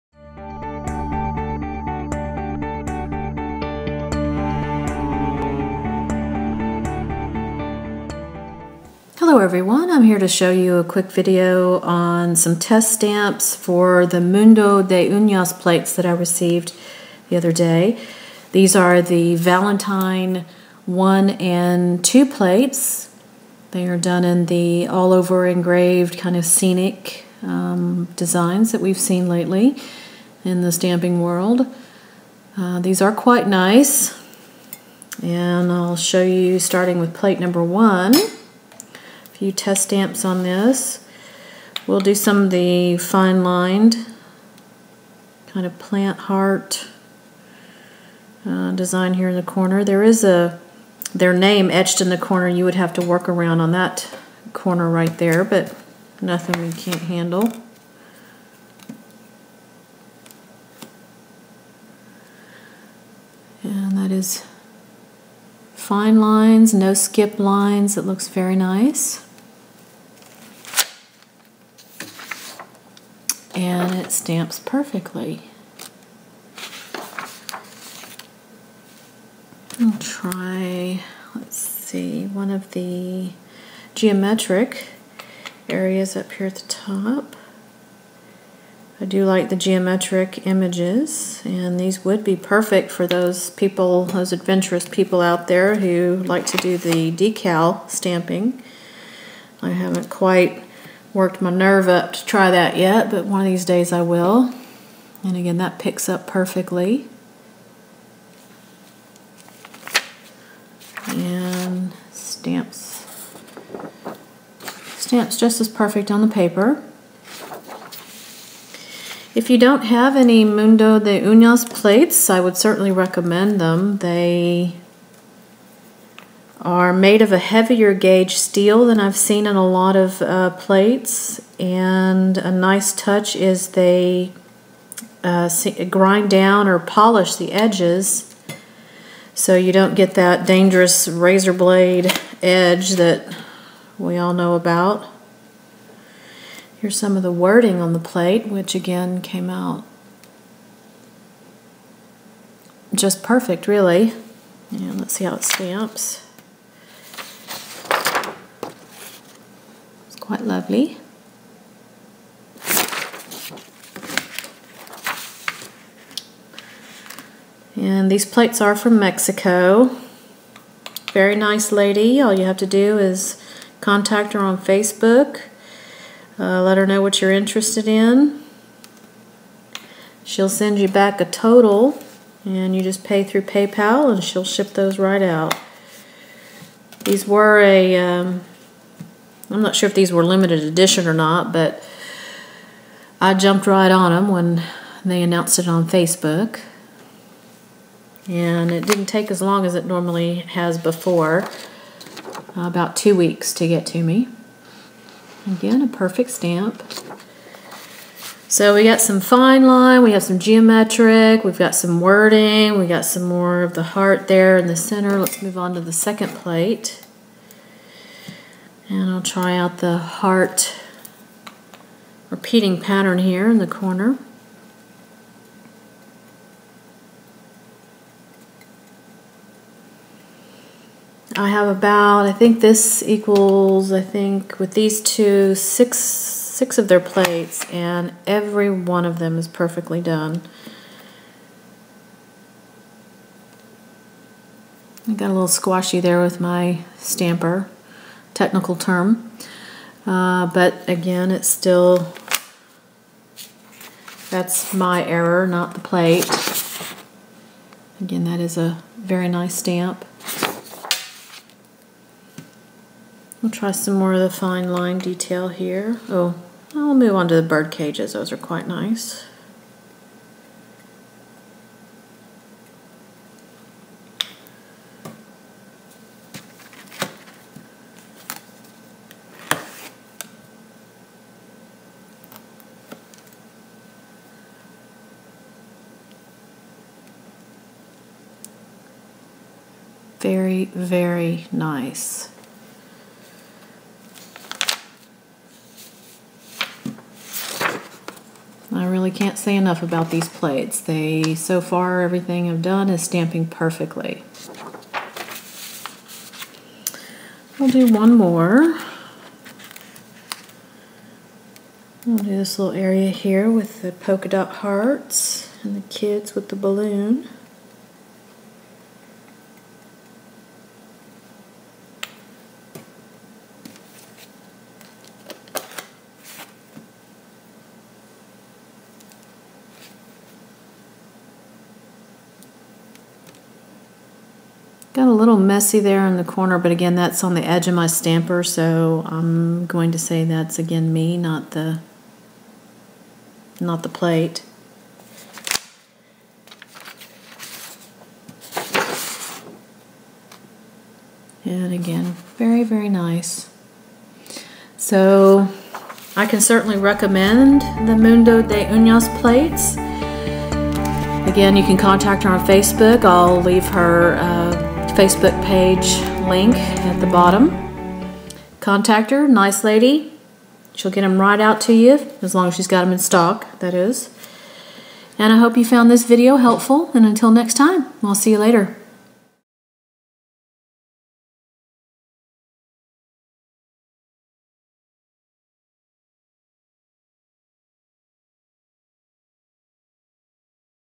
Mundo De Unas Valentine plates test stamps
Mundo V1 an V2 Valentine plates test stamping.